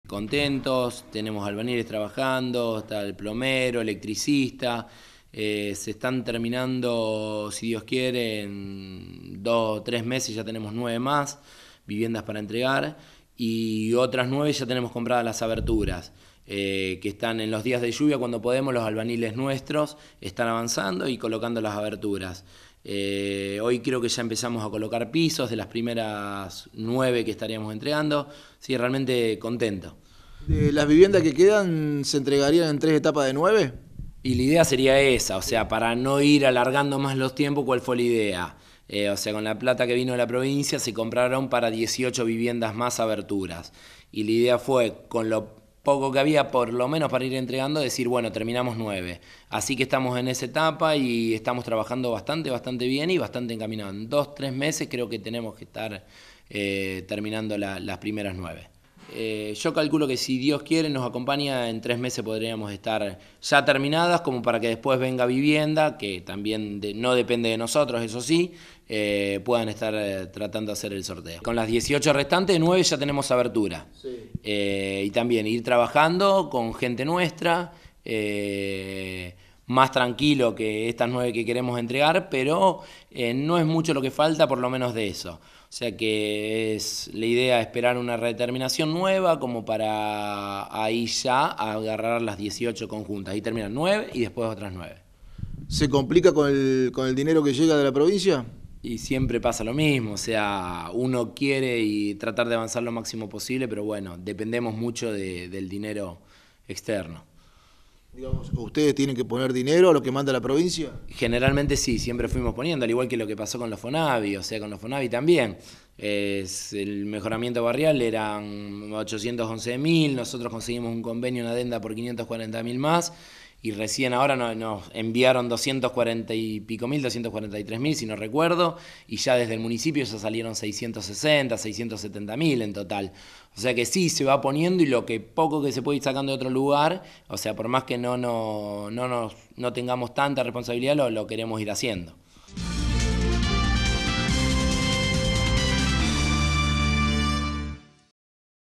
Novedades en el Barrio 36 viviendas
La Municipalidad a través de la Secretaría de Obras Públicas sigue trabajando en el Barrio 36 viviendas, con plomeros, albañiles y electricistas.
Se calcula que en 3 meses se estarán entregando 9 unidades más y para otras 9 ya se están colocando las aberturas.